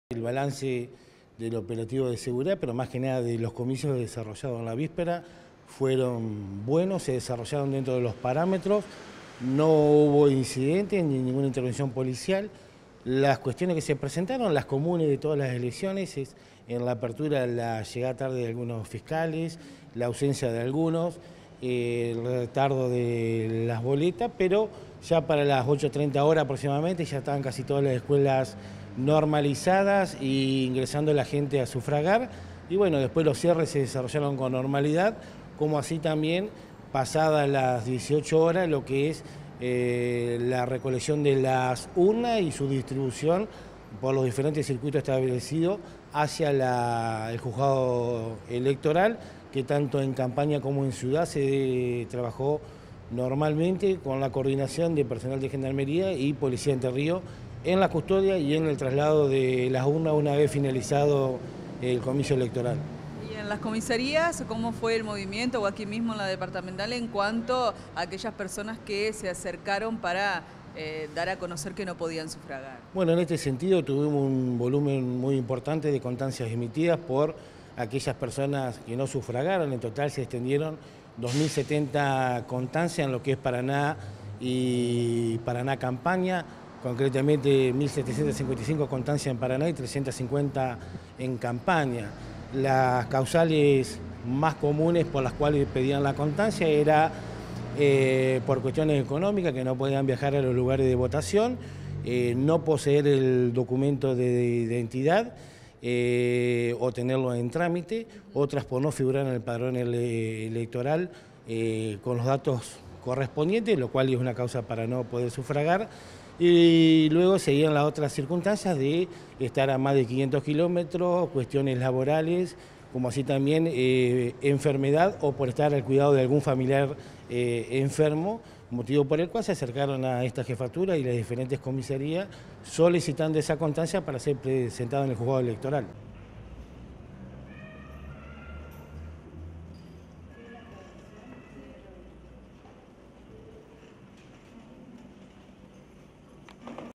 SEGURIDAD EN ELECCIONES Y CONSTANCIAS
MARCOS ANTONIUW
BALANCE OPERATIVO ELECCIONES DE LA POLICIA DE ENTRE RIOS Y TRAMITARON 2700 CONSTANCIAS EN PARANÁ Y PARANÁ CAMPAÑA POR LA NO EMISIÓN DE VOTO
EN DETALLE, FUERON 1755 EN PARANÁ Y 350 EN PARANÁ CAMPAÑA, SUPO ELONCE TV. EL JEFE DE LA DEPARTAMENTAL DE POLICÍA VALORÓ QUE FUE "BUENO" EL OPERATIVO DE SEGURIDAD POR LOS COMICIOS DE ESTE DOMINGO